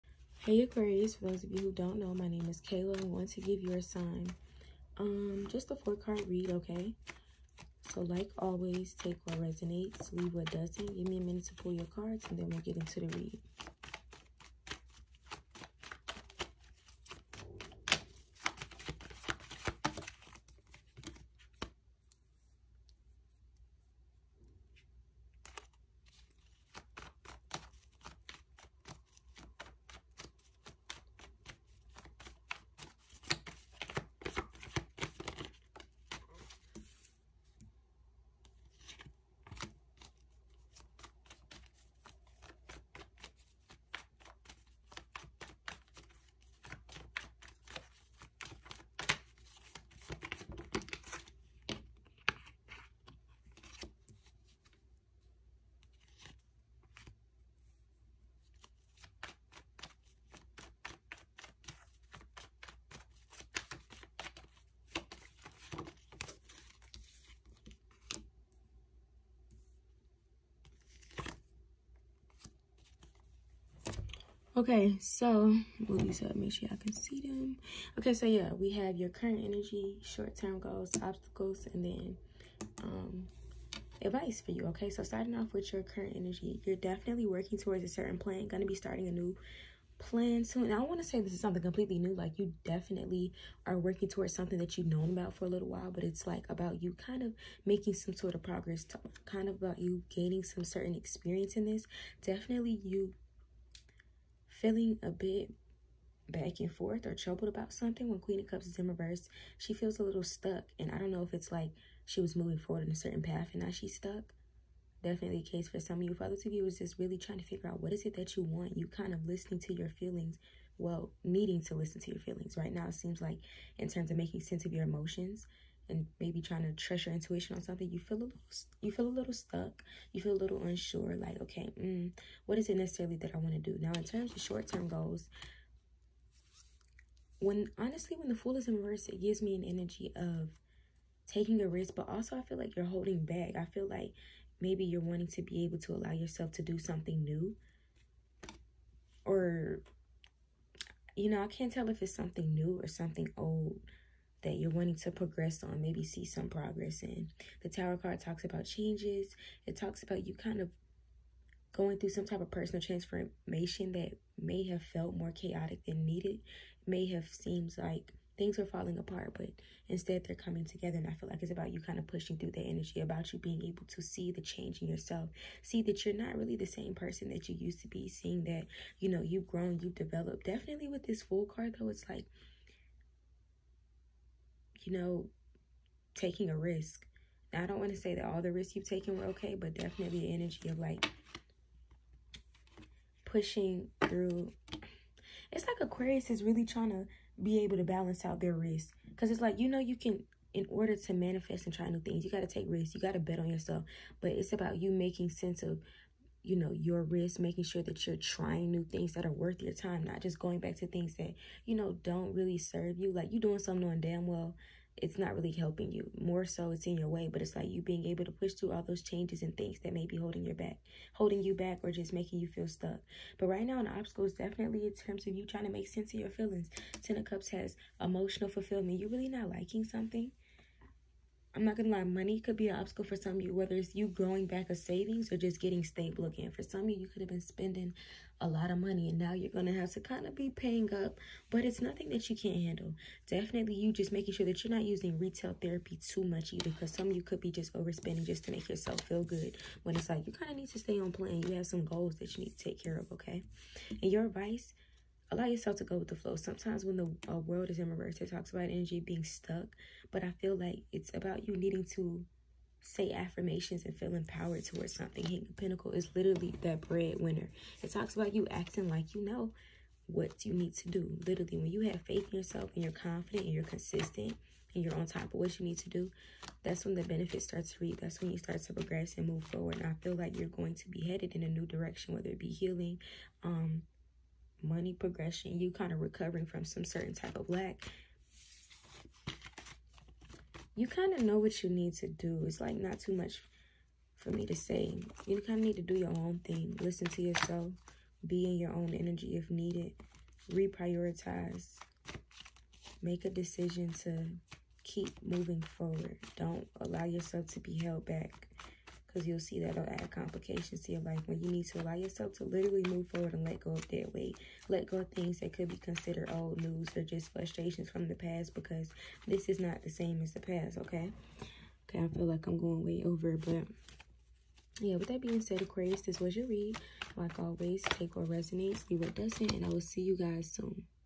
Aquarius Beginning of the Month Read
Aquarius placements reading for the beginning of June. I’m taking a look at your current energy, short term goals for June, obstacles, and advice! Take what resonates and leave the rest.

0:00 Intro & Pulling Cards
1:25 Current Energy
2:13 Short Term Goals
3:43 Obstacles
4:13 Advice